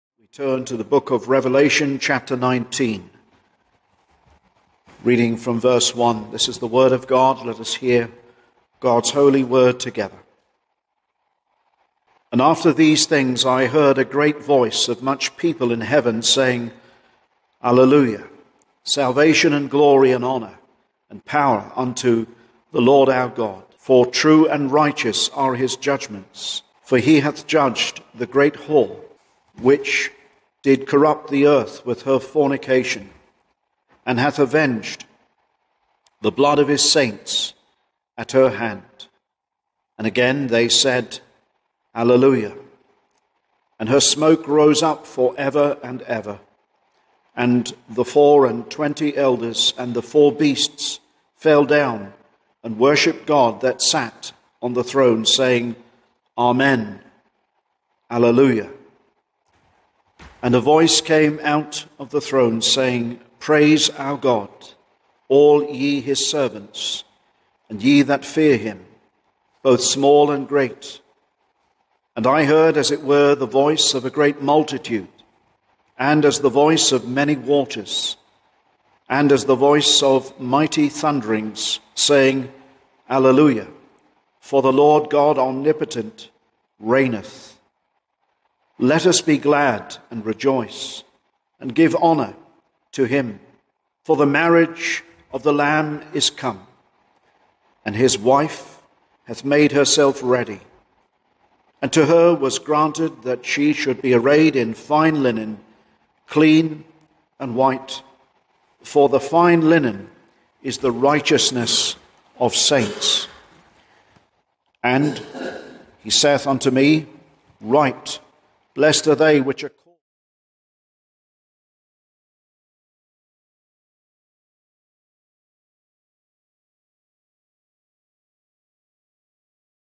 The True Bride revealed as opposed to the Harlot
Here in Revelation 19, is the true Bride of Christ, and especially The Lord Jesus Christ her husband. This follows the description and demise of the Harlot, who is the false Church who claimed to be a queen Revelation 18:7 .